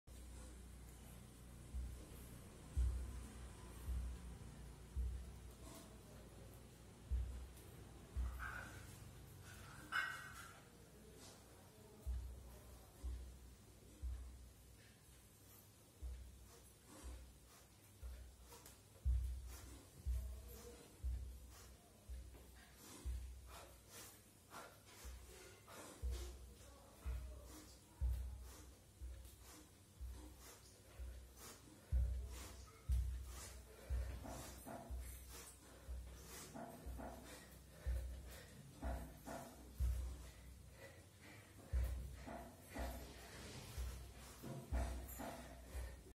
45 reps, Soumyadeep Das, GuruMann Split Squat Challenge 2020
I performed 45 reps in 45 secs.